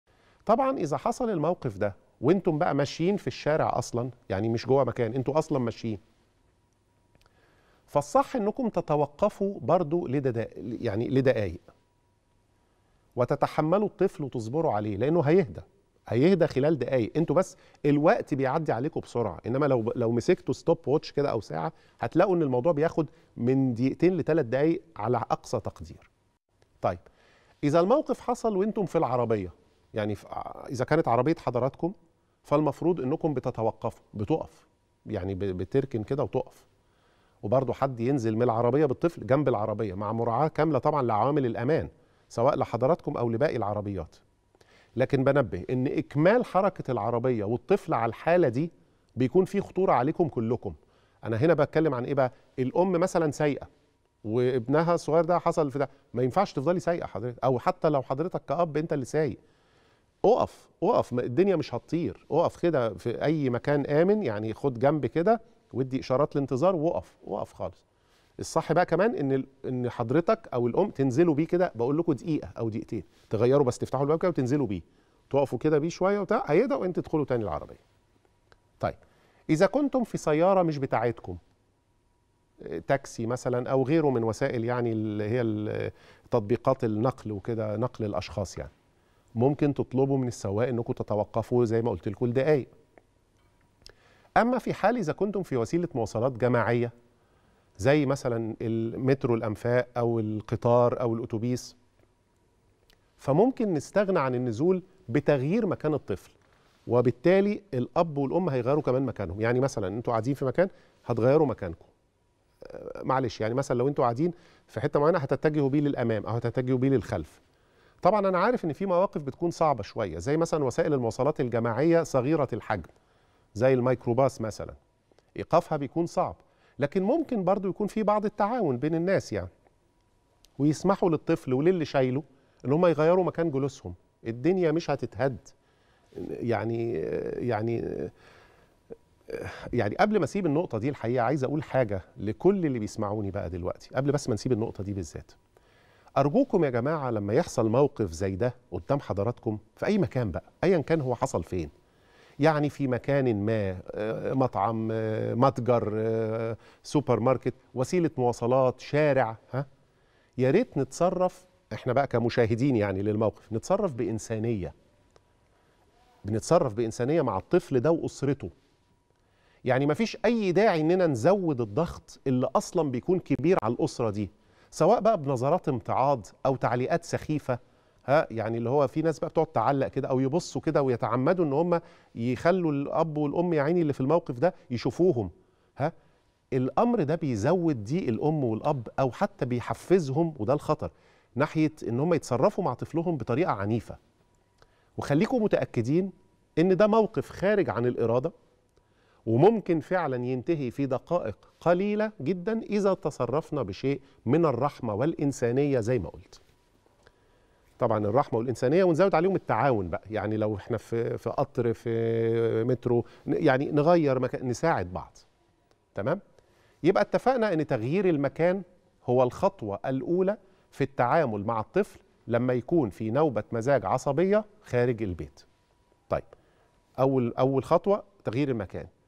لو حصل لابني نوبة مزاج عصبية ومش هاقدر أغير مكاني لأني في العربية مثلا..أعمل إيه؟..د. محمد رفعت يوضح
يحكى أن | لو ابني دخل في نوبة مزاج عصبية ومش هاقدر أغير المكان لأني في العربية مثلاً.. أعمل إيه؟ .. د. محمد رفعت يوضح